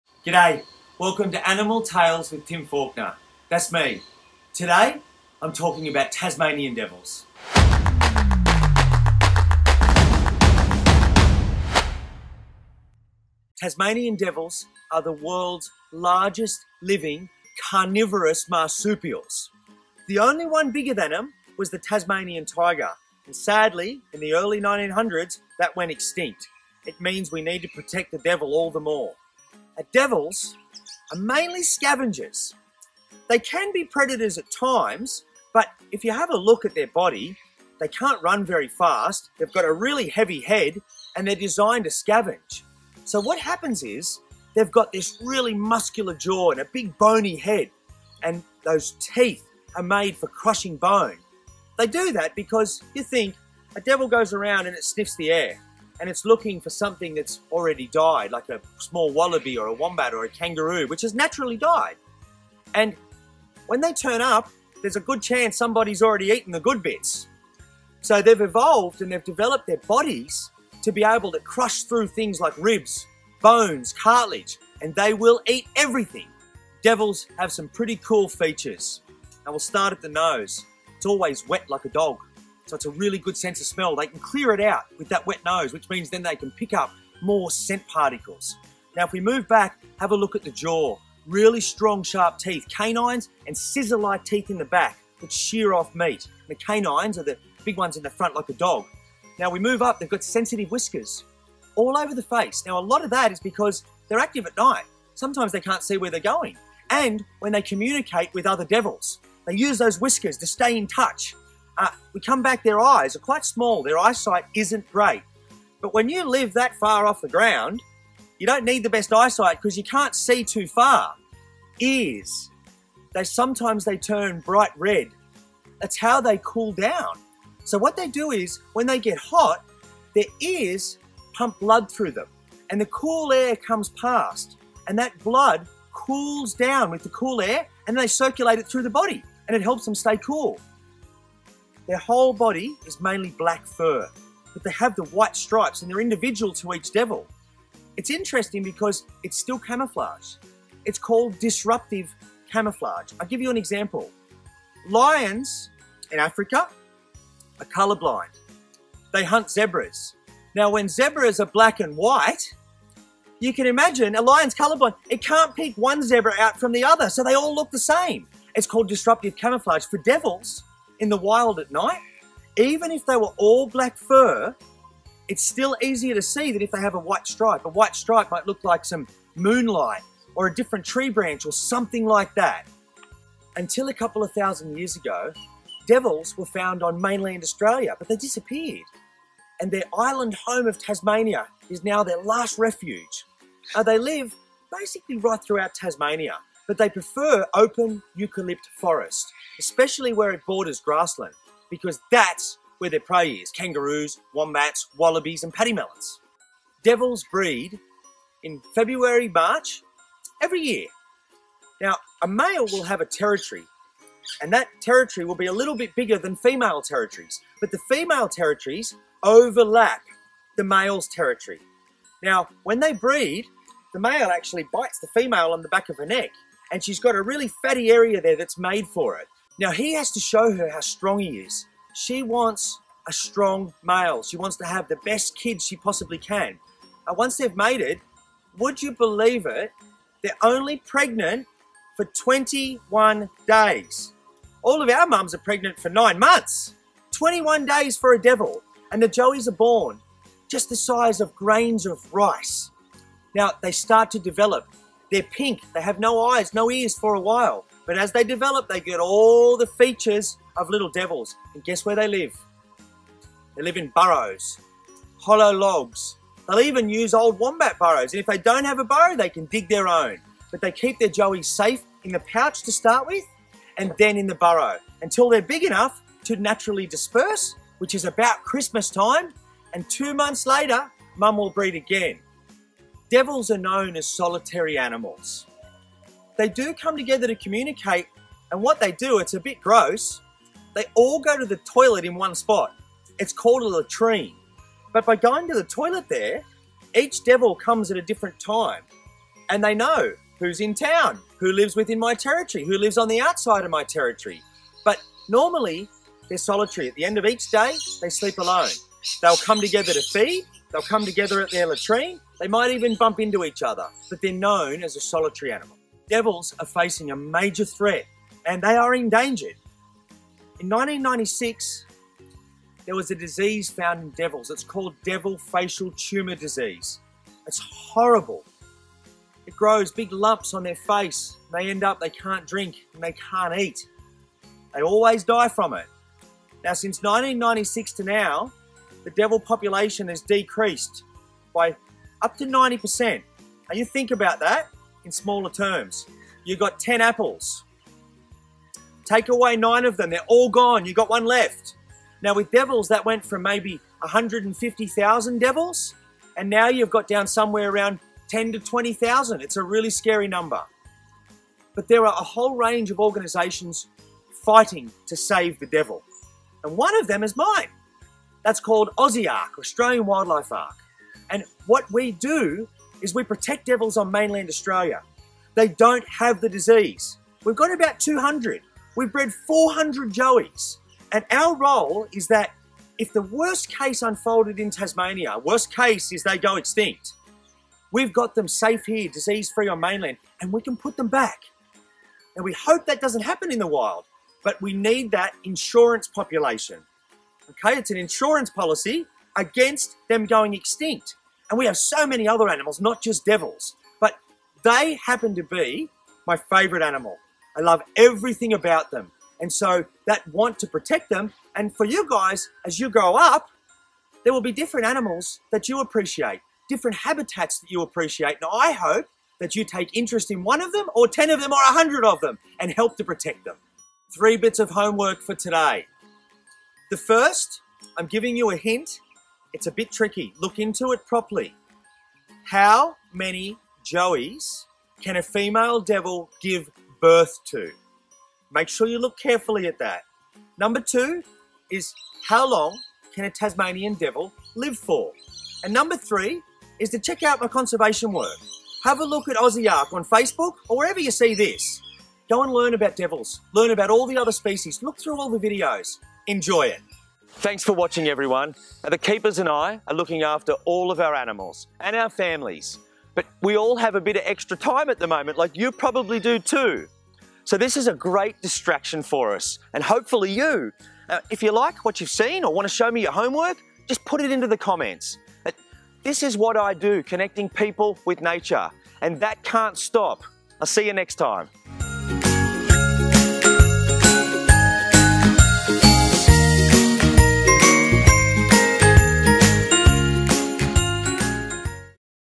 ANIMAL TALES | EPISODE TWO | TASMANIAN DEVILS
🐾ANIMAL TALES WITH TIM FAULKNER! 🐾

In today's episode Tim Faulkner talks about Tasmanian Devils and Conservation!

Don't forget to post your homework in the comments below! 👇

❌CONTENT ADVISORY: This episode features Tasmanian Devils eating raw meat and two images of Devil facial tumor disease ❌